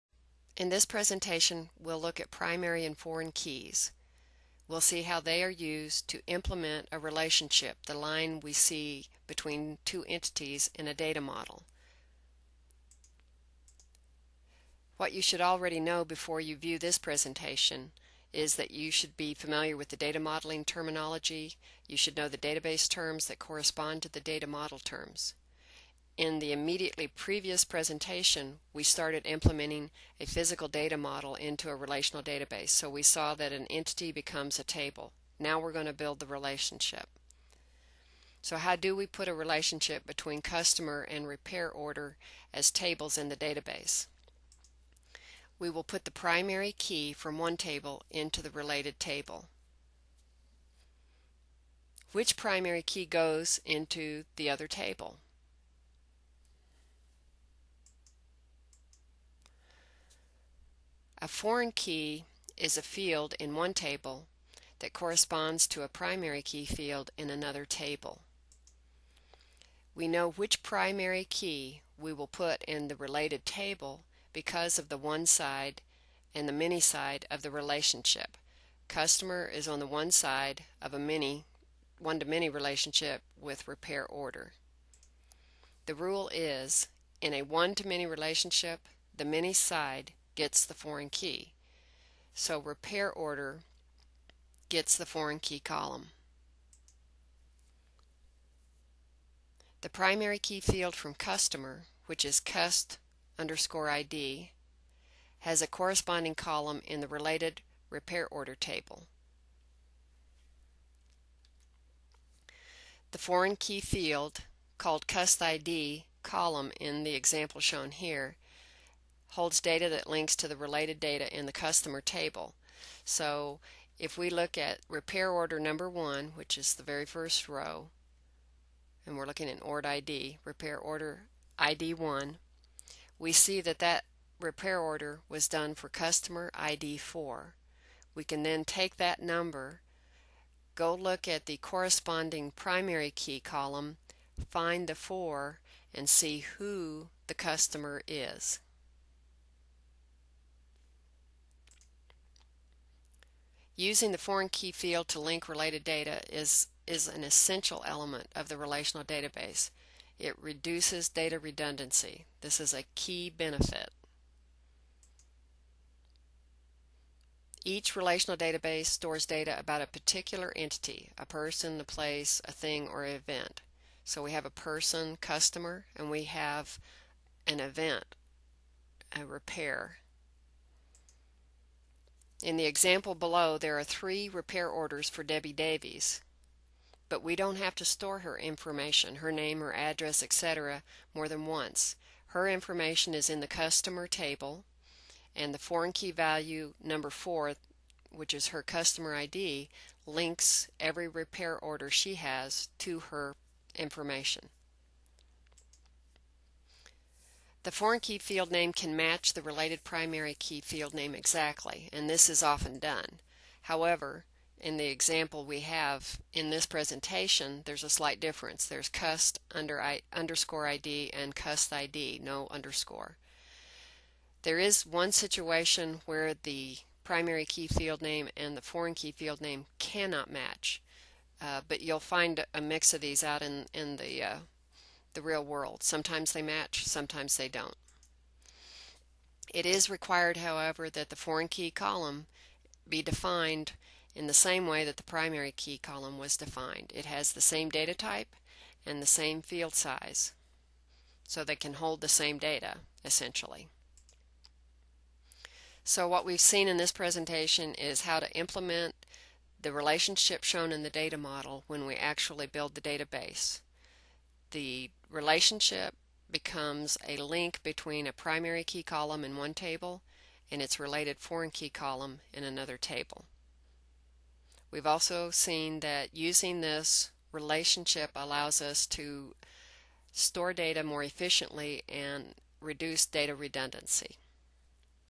MEC Database Fundamentals (06 of 10): Physical Design - Primary & foreign keys
This database tutorial covers how relationships are implemented in a relational database.  Primary keys and foreign keys are explained.  This is part of a series of videos about relational database concepts. Part  6 of 10.